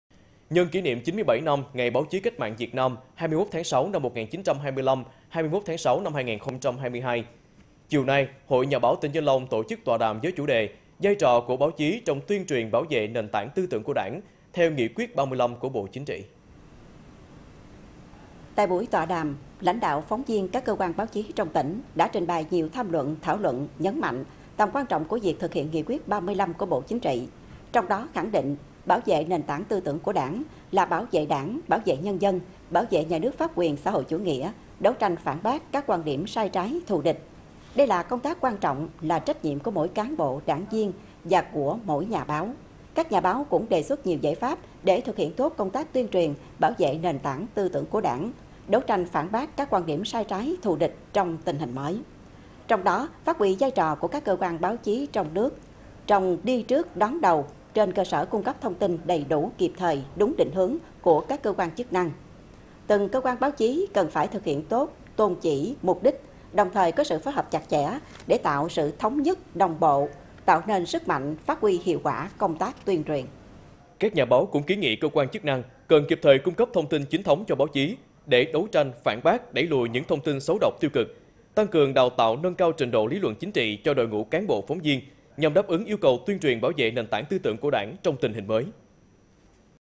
Hội Nhà báo tỉnh Vĩnh Long tọa đàm "Vai trò của báo chí trong bảo vệ nền tảng tư tưởng của Đảng"
Nhân kỉ niệm 97 năm ngày Báo chí Cách mạng Việt Nam (21/6/1925  – 21/6/2022), chiều 15/06, Hội Nhà báo tỉnh Vĩnh Long tổ chức tọa đàm với chủ đề: “Vai trò của báo chí trong tuyên truyền bảo vệ nền tảng tư tưởng của Đảng” theo nghị quyết 35 của Bộ Chính trị .